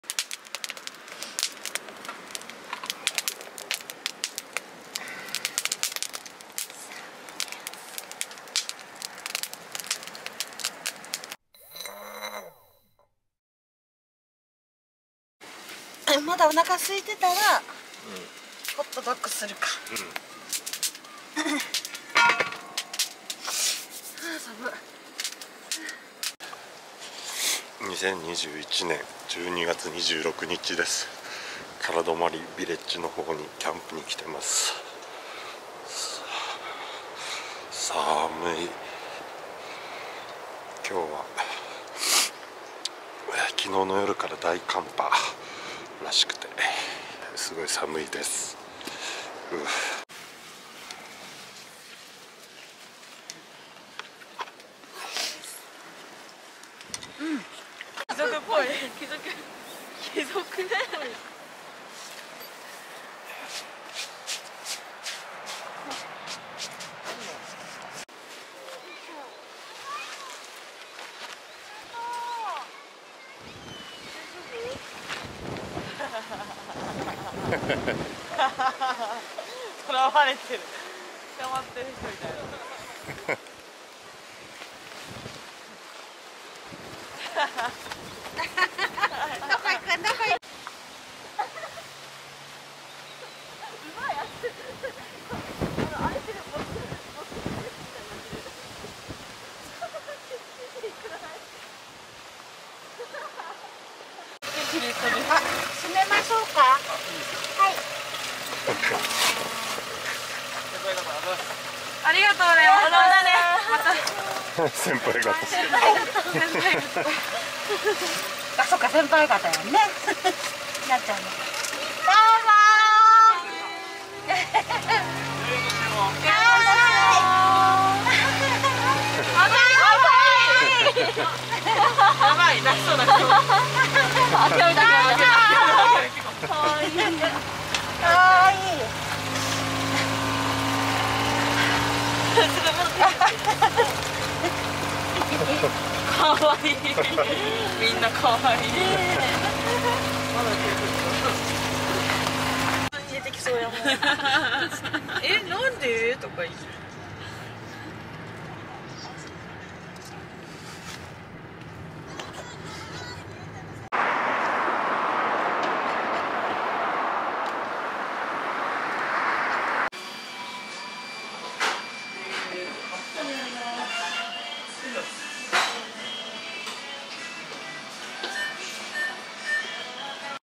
唐泊Villageでキャンプ / Dec. 26, 2021 / Masa's Daily Vlog
I take a video of my normal life every day, edit it, and upload it to YouTube. My only ingenuity is that the video will not be published for two years from uploaded. This daily vlog was taken 2 years ago, on Dec. 25, 2021.
Enjoyed camping with the wife, daughter, and her friends.

毎日、自分の普段の生活を動画で撮影し、編集してYouTubeにアップロードしているのです。唯一の工夫は、アップロードしてから2年間は動画が公開されないことです。このデイリーVlogは2年前の2021年12月26日に撮影したものです。
妻と娘とその友人とキャンプを楽しんだ。